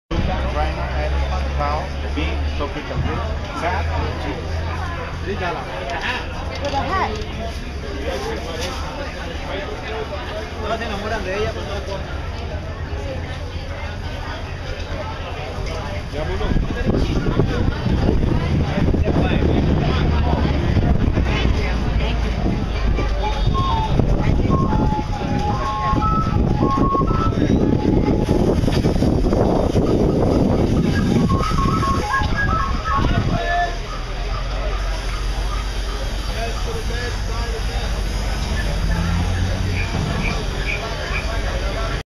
Costa Rica Souvenir Store
Stop by the souvenir store t at the quayside in Limon, Costa Rica.

The arts & crafts are beautiful and made in Limon, Costa Rica.